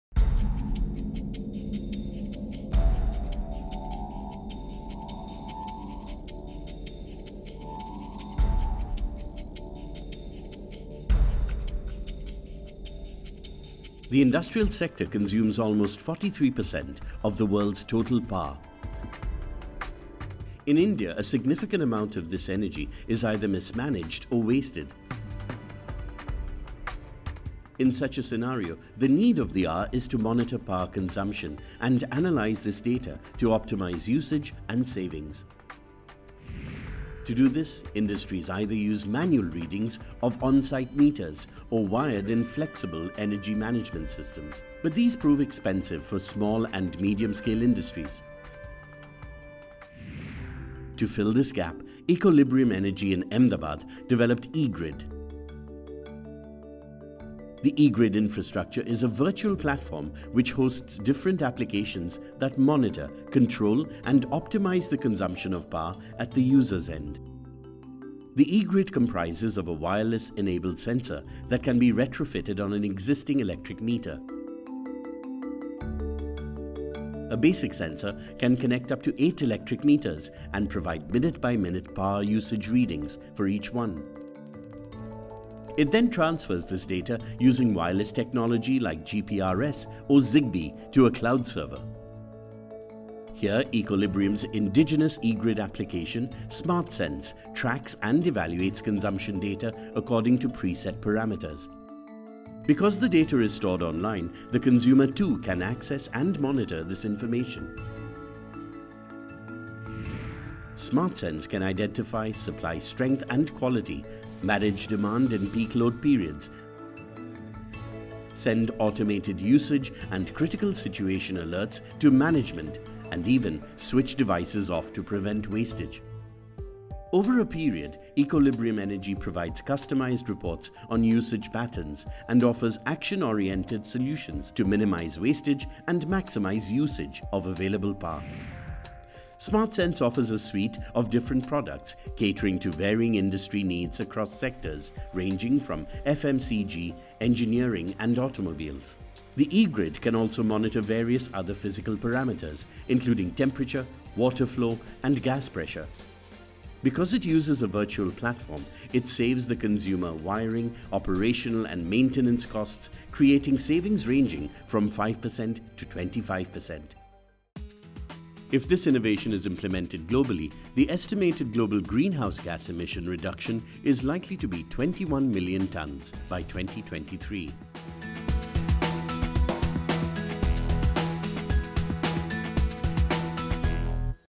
E Grid: Low-Cost Platform and Analytics for Demand Side Management by Ecolibrium Energy Pvt. Ltd
Winner of the Climate Solver 2013 Awards:
GHG Reduction